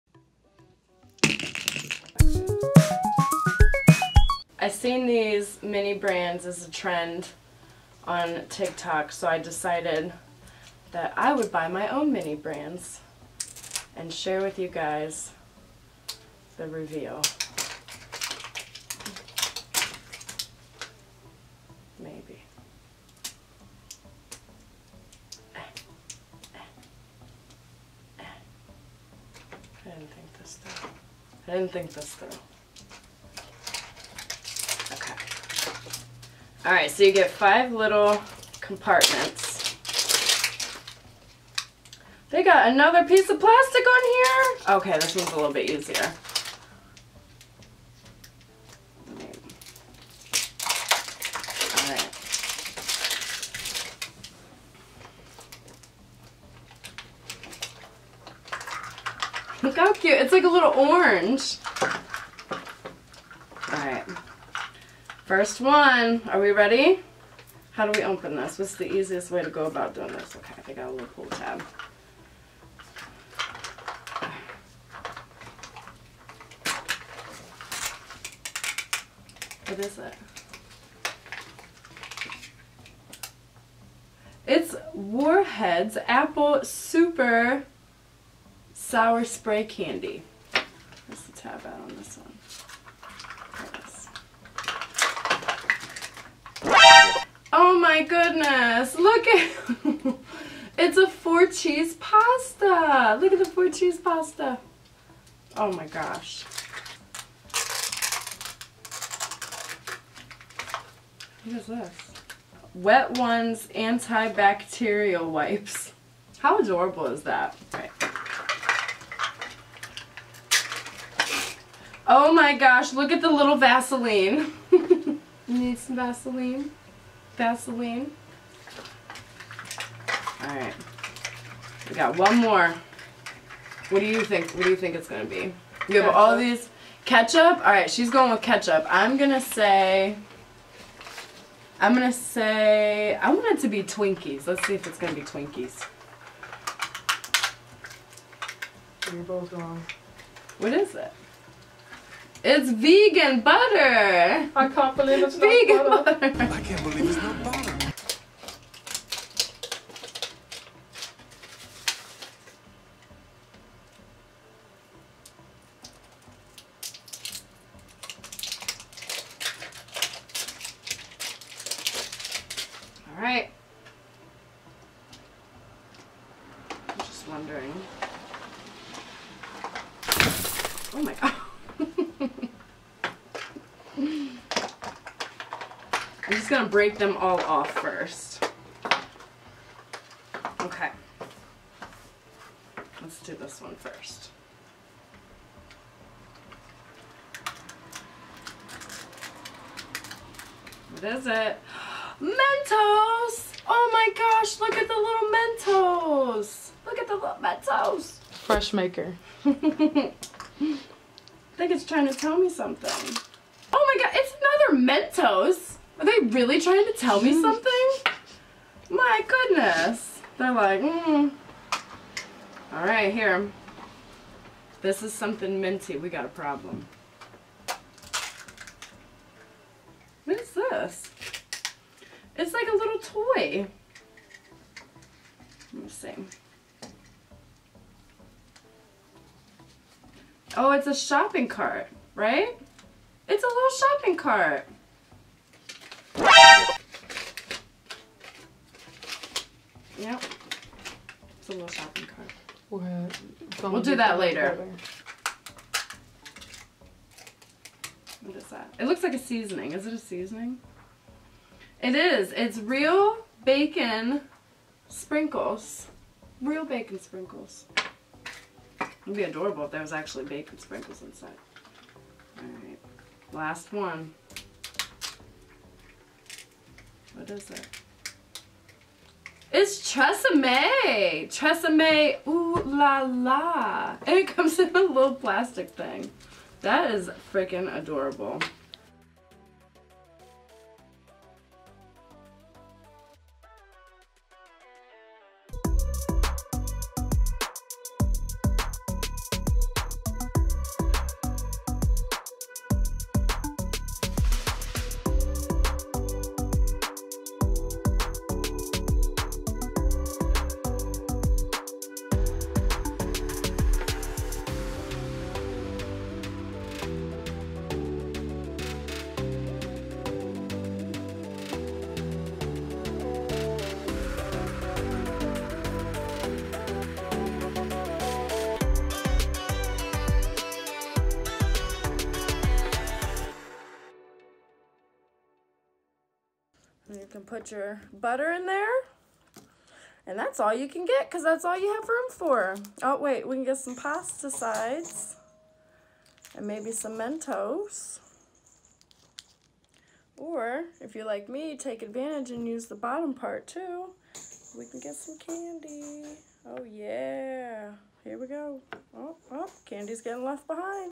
My First Unboxing: Mini Brands by Zuru *ASMR* edition
You won't believe what we find inside my Mystery Ball. I am trying to stock my mini home. I won't stop opening them until I have every last Mini Brand!!! Which ones will we get today? With a whopping 70+ miniature brands to collect who knows how long it will take to complete my collection.

My Top Ten Mini Brands Wish List:

Skippy Peanut Butter
Bryers Chocolate Ice Cream
Mentos Candy
Lipton Green Tea
Airheads Candy
SPAM Lite
Helmans Mayonaise
Dove Bodywash
TRESsemme Shampoo
WetOnes Wipes

My Gear:
Camera: Canon t4i
My Microphone: Rode NTG 2
My recorder: Zoom H4N Pro